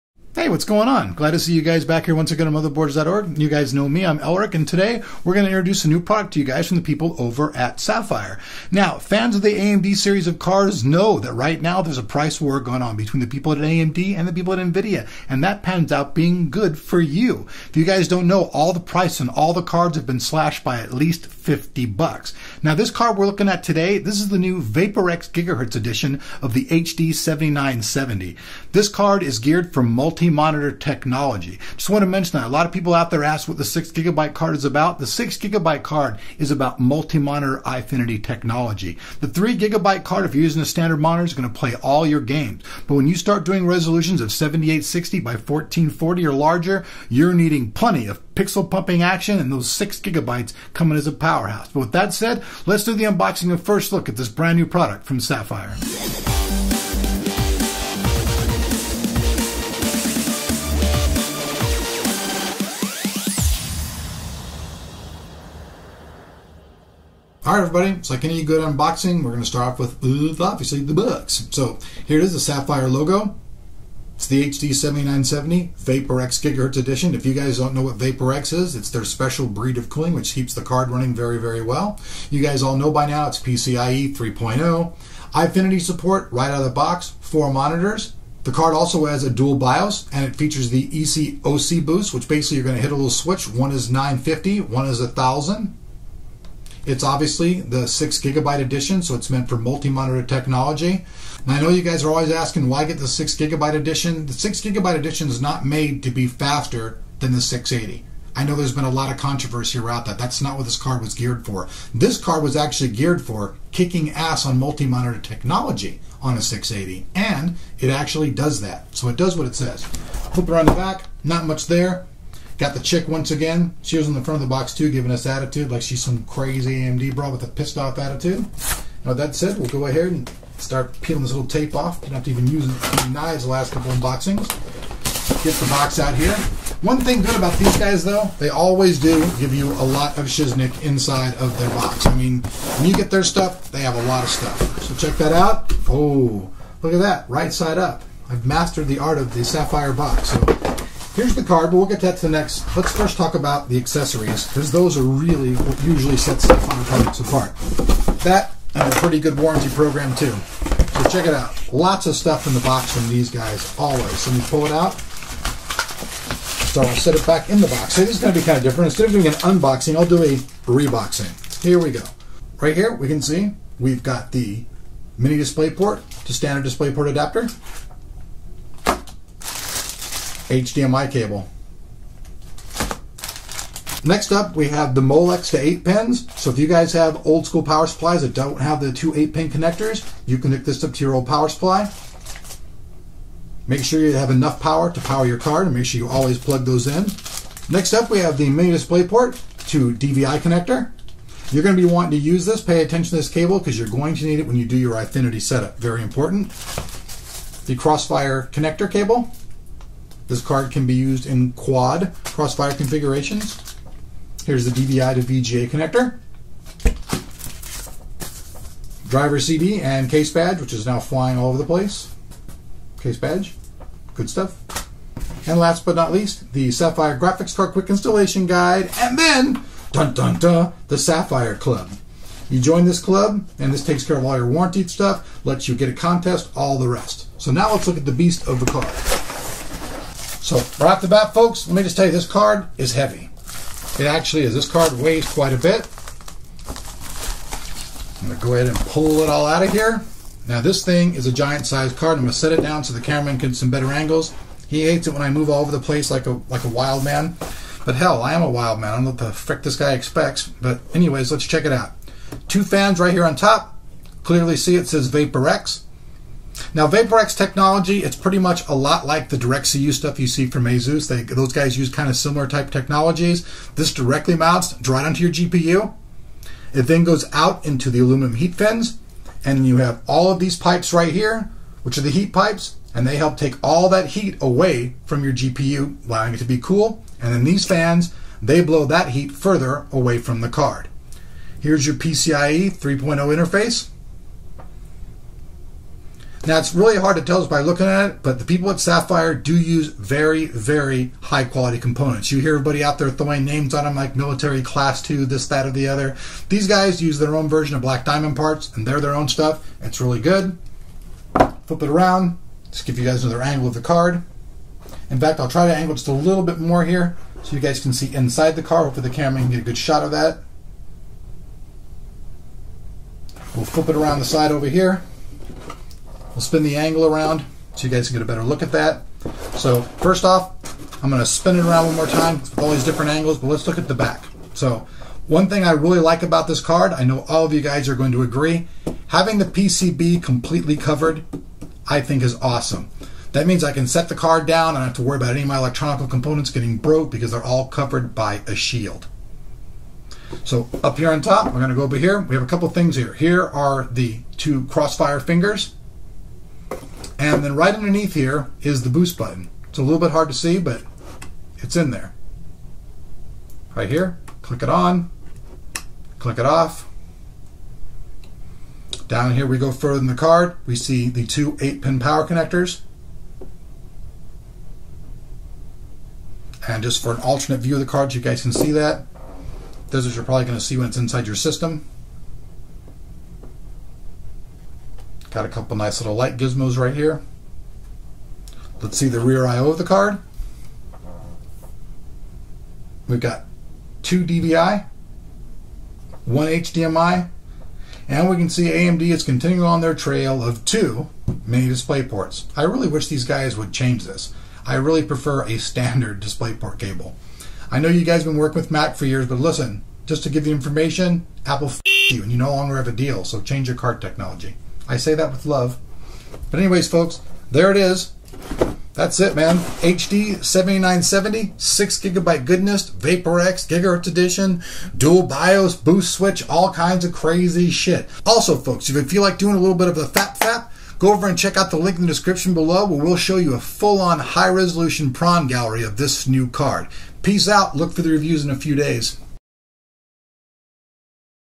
Sapphire 6GB HD 7970 Vapor-X GHz Edition Video Card Unboxing
Product.

1050/1100(Boost) MHz Core Clock
6000 MHz Effective Memory Clock
1 x Dual-Link DVI
1 x Single-Link DVI-D
1 x HDMI
2 x Mini-DisplayPort
DisplayPort 1.2

Never settle for less with SAPPHIRE's HD 7970 Vapor-X 6GB OC Edition graphics card. Driven by the power of AMD's next-generation 28-nm GCN architecture and 6GB of memory, you can take your settings to the max, devastate your enemies and witness absurdly high frame rates—DirectX 11 has never looked this good. Pair all this raw power with SAPPHIRE's Vapor-X technology and you have a blazing fast ticket to experience gaming like never before - all at cool temperatures and with low noise. OC Editions are factory overclocked right out of the box -- maximize your performance even more, using TriXX, SAPPHIRE's exclusive overclocking tool. TriXX allows tuning of GPU voltage as well as core and memory clocks, while continuously displaying temperature.

Credit: motherboardsorg